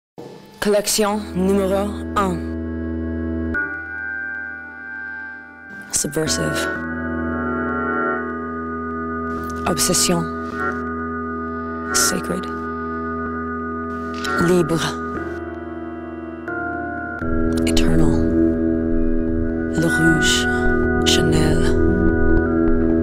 Chanel Le Rouge Collection N°1
Advertisement for the new Chanel Le Rouge Collection N°1 starring Kristen Stewart.
Thanks to Chanel China Tencent Channel and @CiderFlood